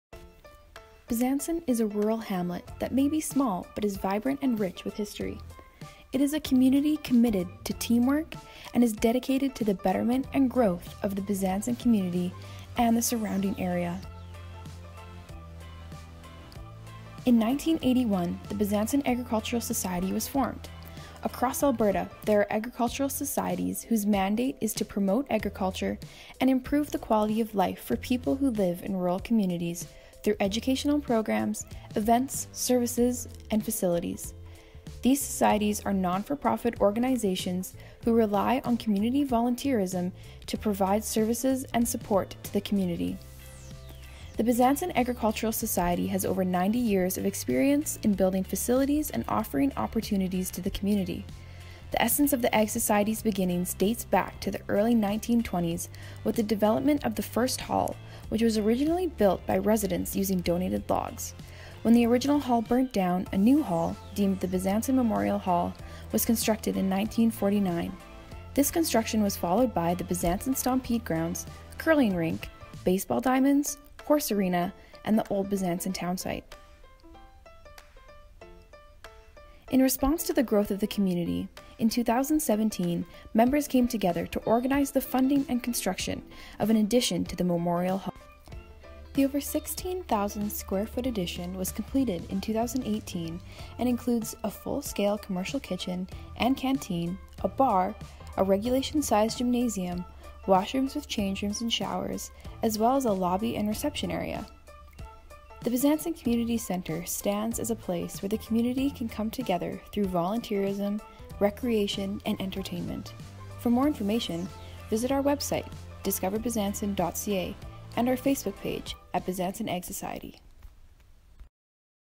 History of the Bezanson Ag Society
Learn all about the history of the Bezanson Ag Society! Our current facilities include the operation and rental of two commercial kitchens, the Memorial Hall, and a regulation size gymnasium.